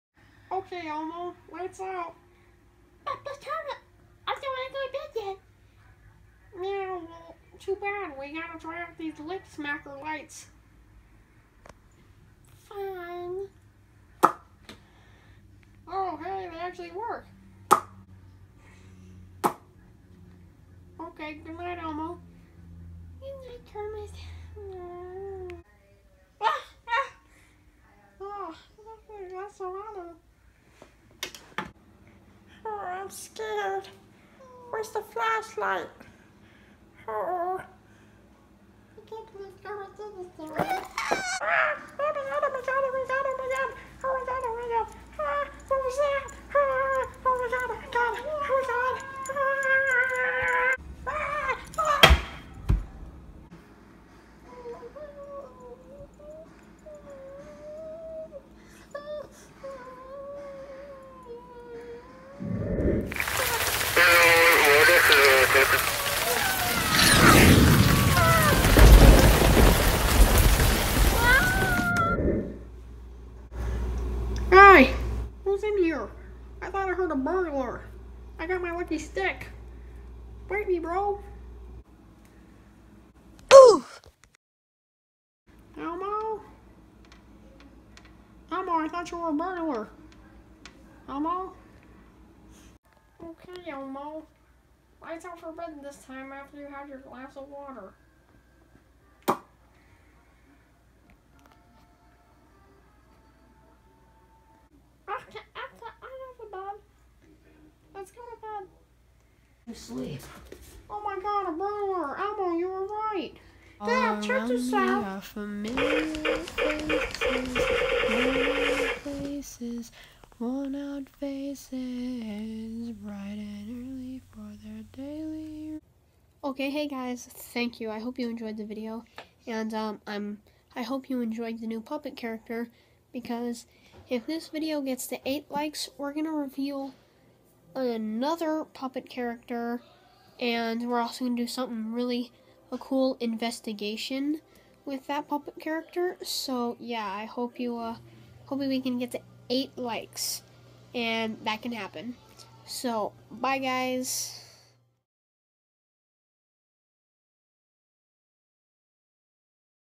Elmo is scared of the dark and Kermit gets bossy/ can we get 8 likes
its all in the title